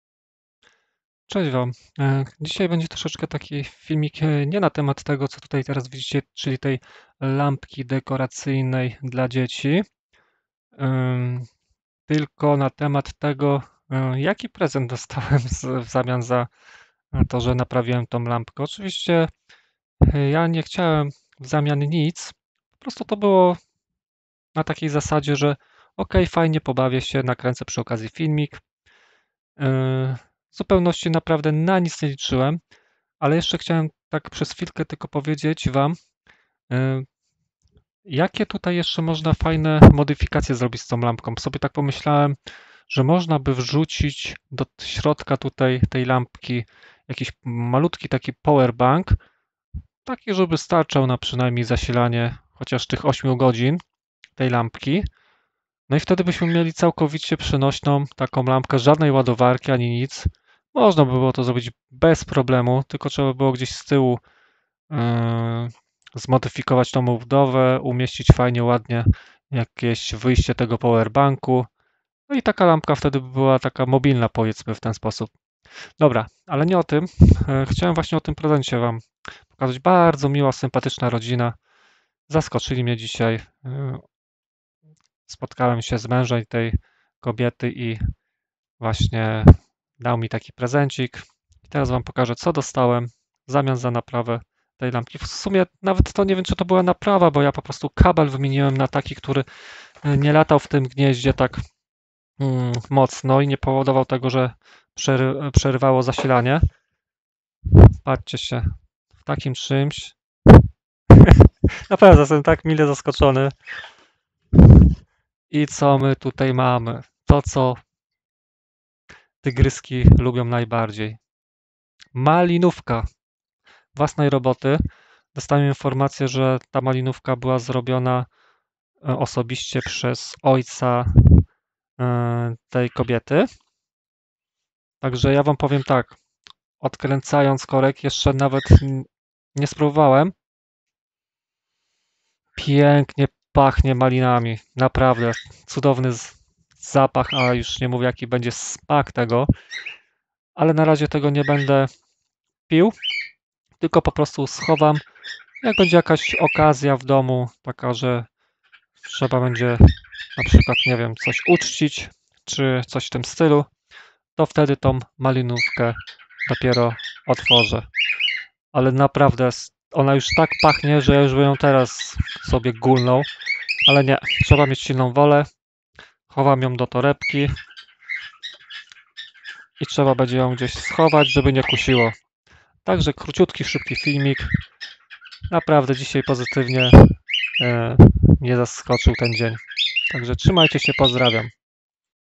Prezent :)
Dzisiaj mile zaskoczenie...w prezencie za naprawę lampki dostałem cala buteleczkę własnoręcznie robionej malinówki :) Pachnie przepięknie...aż by się chciało pociągnąć z gwinta hehe ale zostawię na specjalna okazje.